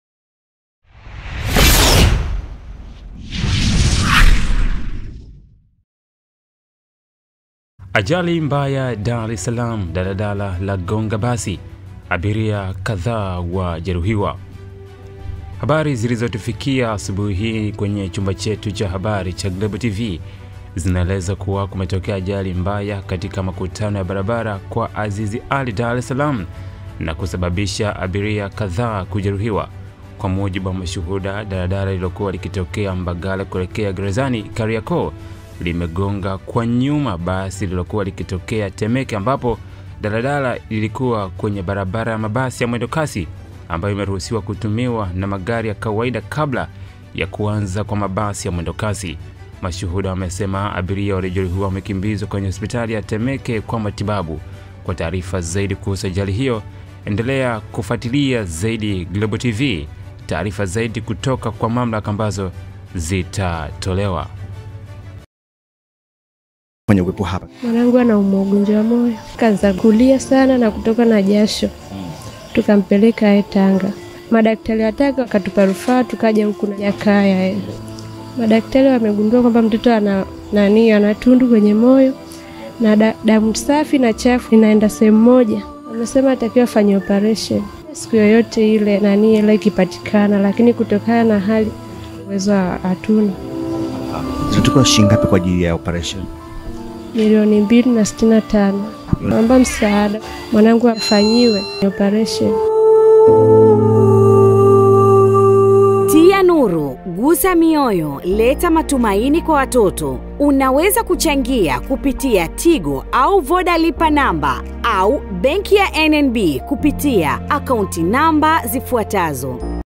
BREAKING: AJALI MBAYA DAR KWENYE BARABARA ya MWENDOKASI - DALADALA LAGONGA BASI - KADHAA WAJERUHIWA
💪💯JIUNGE NA GROUP LETU LA WHATSAPP UWE MWANAFAMILIA WA GLOBAL TV🔥🔥